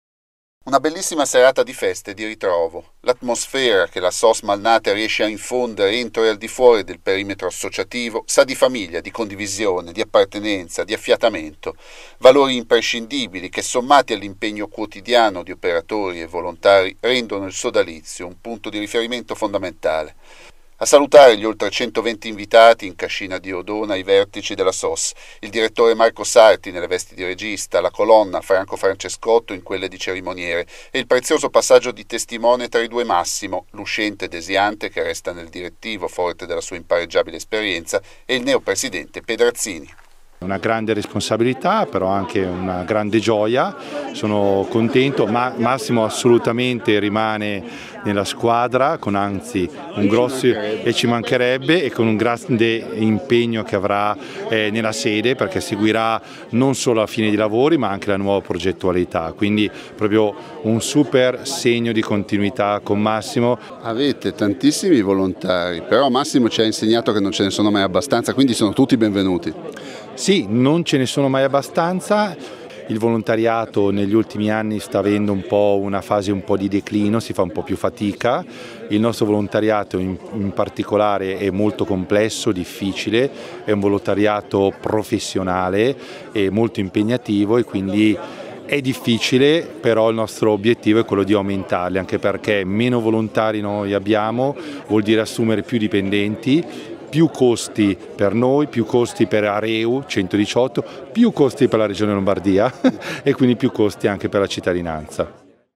Malnate - SOS in festa, staffetta tra i due “Massimo”
Una bellissima serata di festa e di ritrovo.
 L’atmosfera che la SOS Malnate riesce a infondere entro e al di fuori del perimetro associativo sa di famiglia, di condivisione, di appartenenza e di affiatamento.
Valori imprescindibili che sommati all’impegno quotidiano di operatori e volontari rendono il sodalizio un punto di riferimento imprescindibile.
A salutare gli oltre 120 invitati alla Cascina Diodona, i vertici di SOS: il direttore Marco Sarti nelle vesti di regista, la colonna Franco Francescotto in quelle di cerimoniere, e il prezioso passaggio di testimone tra i due Massimo, l’uscente Desiante, che resta nel direttivo forte della sua impareggiabile esperienza, e il neo presidente Pedrazzini.